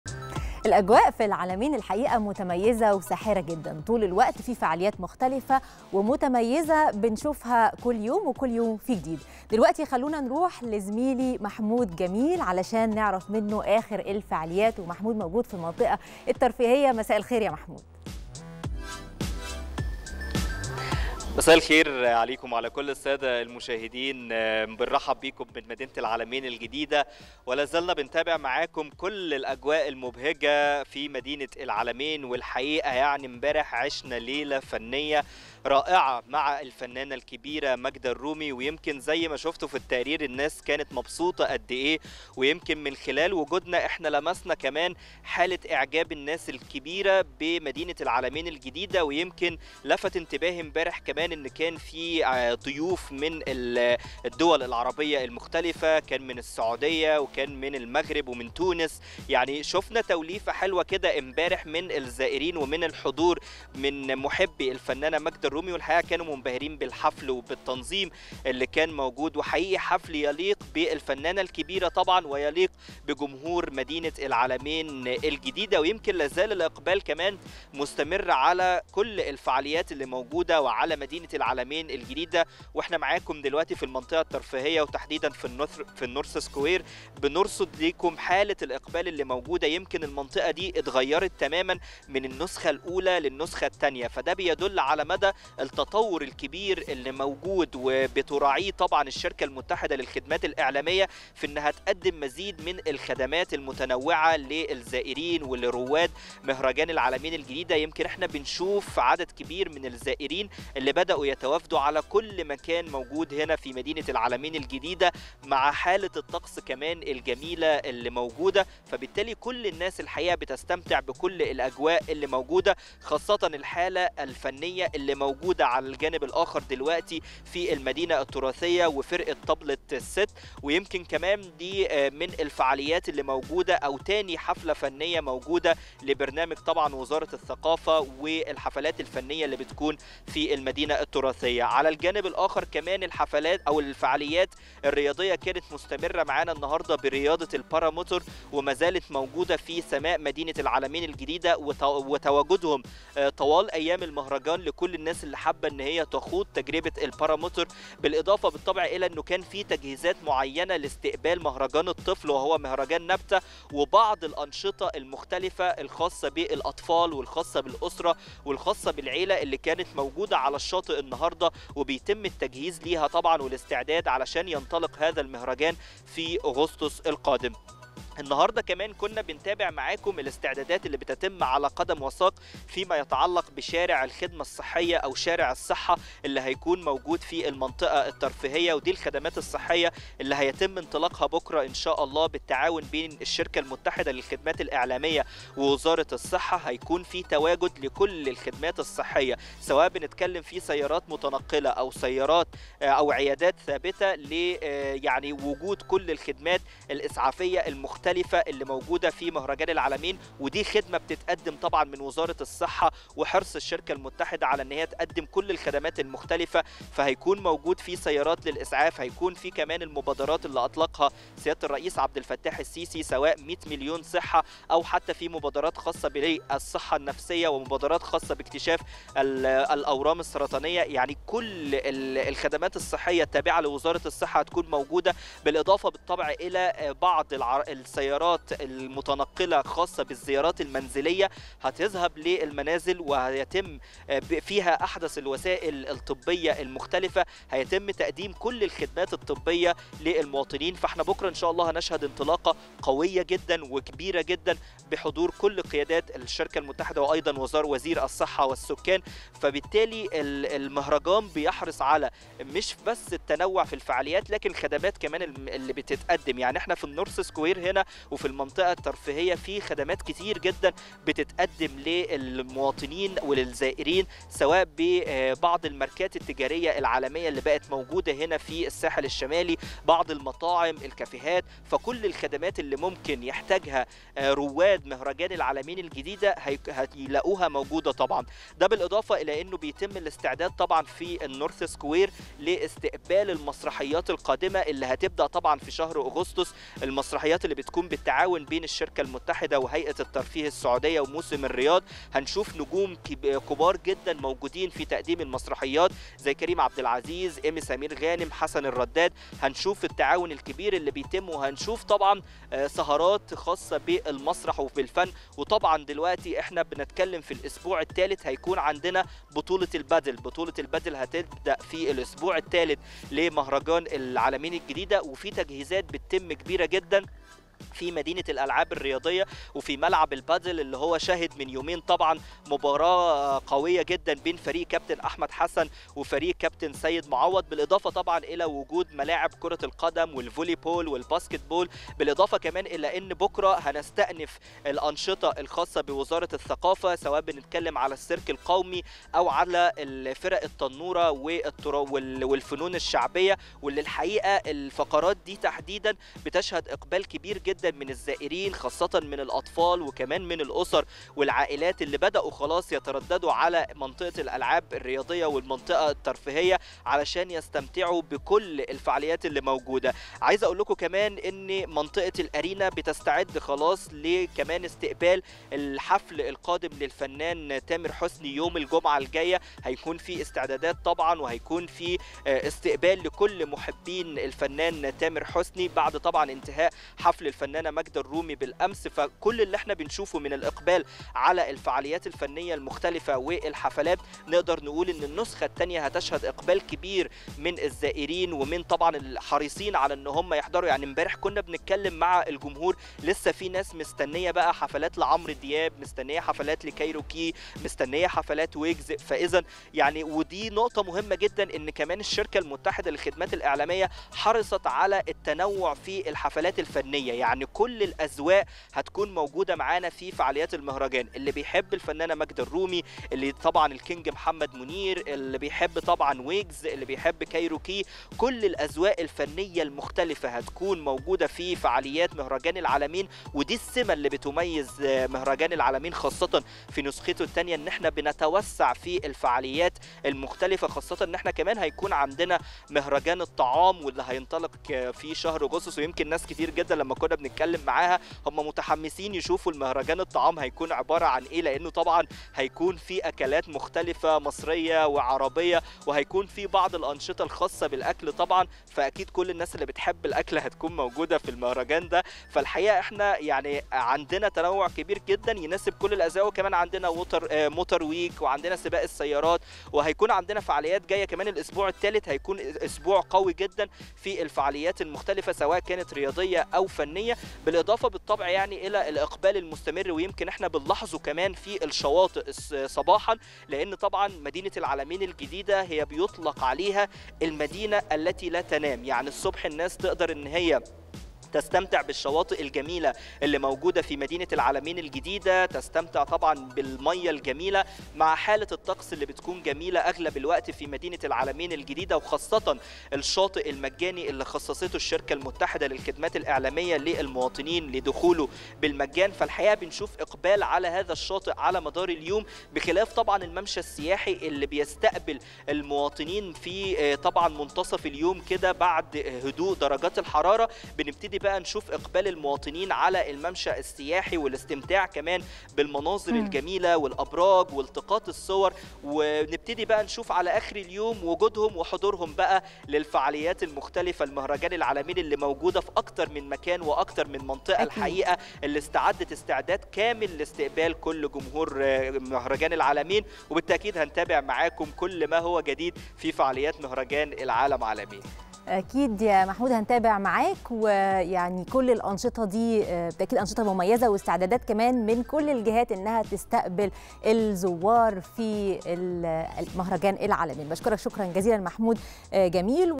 الأجواء في العلمين متميزة وساحرة جدا... من جوا المنطقة الترفيهية هتشوف أجمل الفعاليات ليك وصحابك
الأجواء في العلمين متميزة وساحرة جدا... من جوا المنطقة الترفيهية هتشوف أجمل الفعاليات ليك ولأطفالك وأنت وصحابك